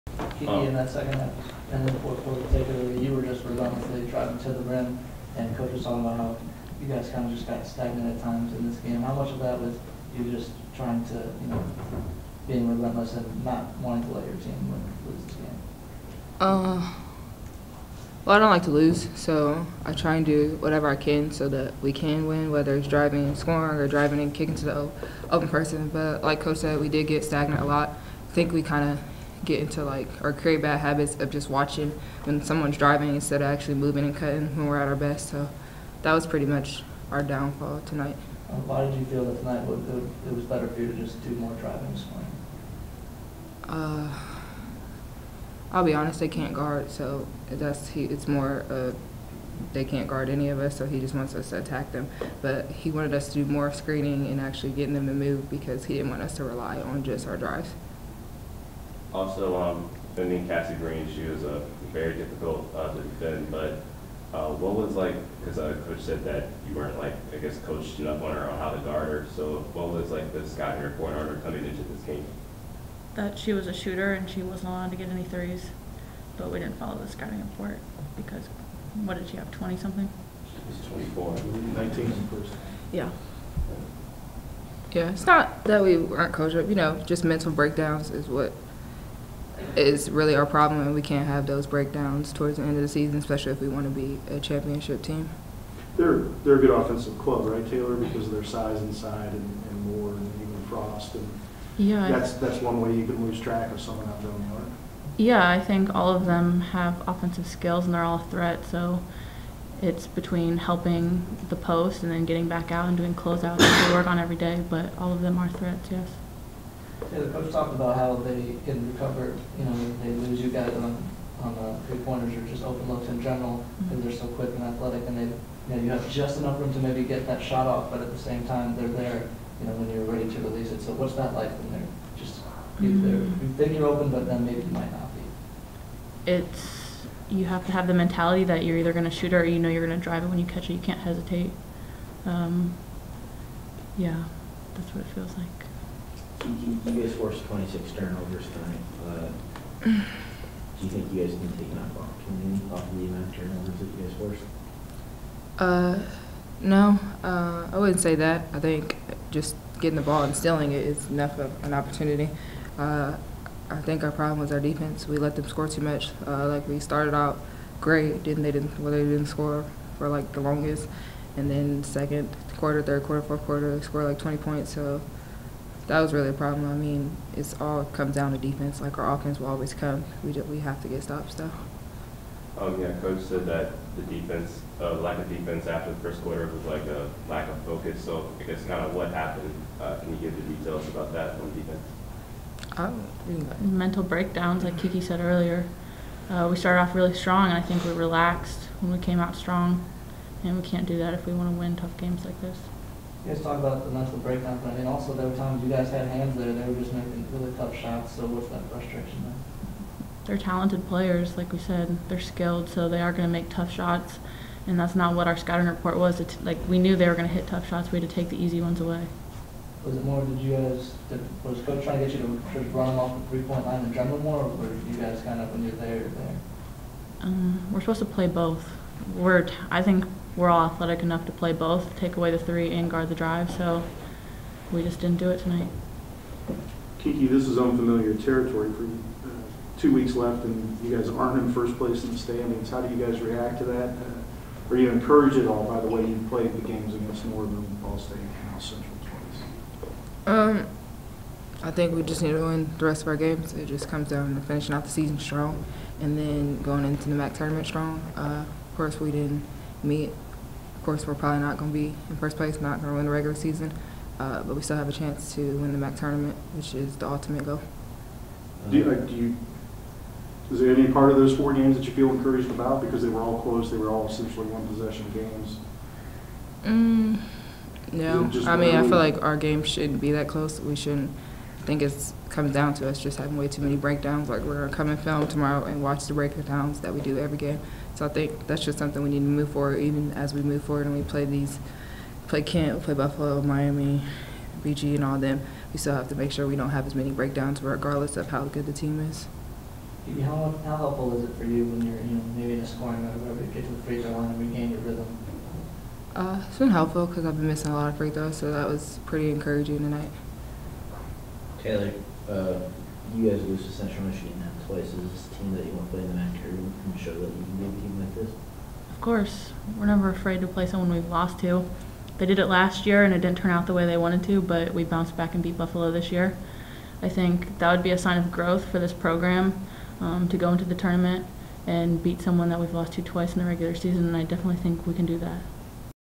Ohio Women's Basketball 2017: Taylor Agler and Quiera Lampkins post CMU
Taylor Agler and Quiera Lampkins speak with the media after the Bobcats 70-64 loss to CMU on Feb 15, 2017